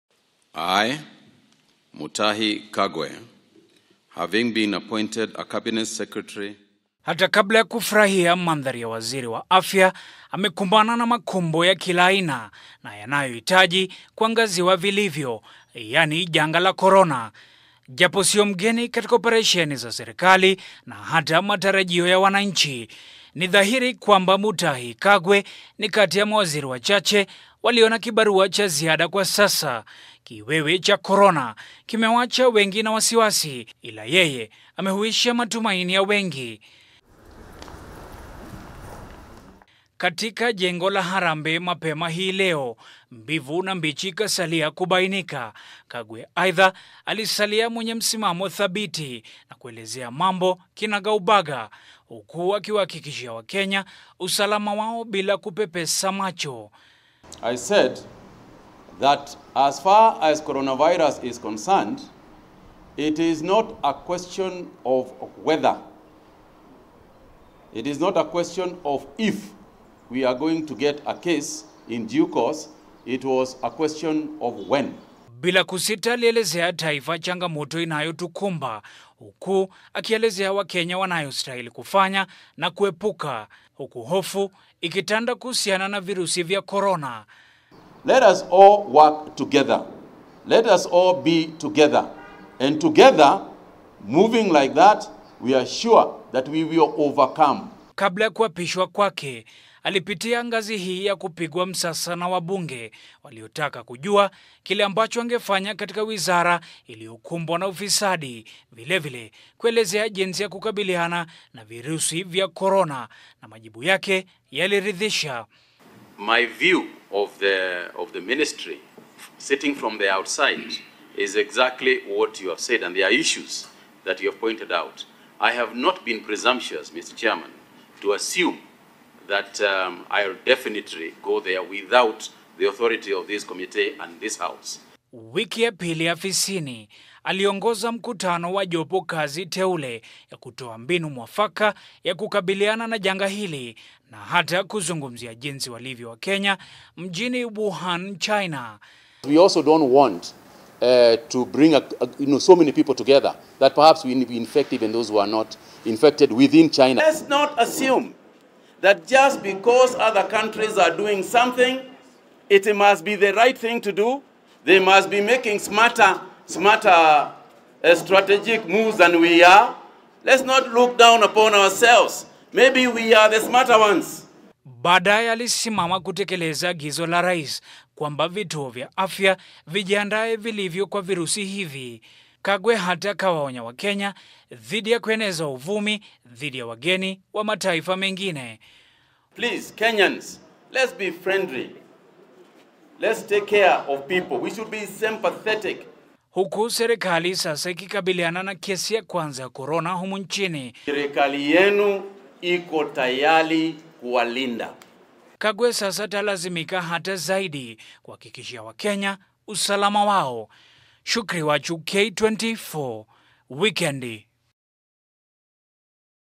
Ukakamavu wa Mutahi Kagwe
Tangu, kuteuliwa kwake , kupigwa msasa na hatimaye kuapishwa kama waziri wa afya, aliyekuwa seneta wa Nyeri Mutahi Kagwe bila shaka ni mmoja kati ya wachache waliopitia anachokipitia kwa sasa siku kadha tangu kuanza kazi yake rasmi.
Lakini japo ya hilo, ni waziri ambaye usemi wake, na jinsi anavyoelezea mambo anawaacha wengi na matumaini japo ya janga kama hili la Corona.
Mwahabari wetu Shukri Wachu sasa anatueleza mengi kuhusu Mutahi Kagwe, siku 15 tangu kuwa waziri wa afya.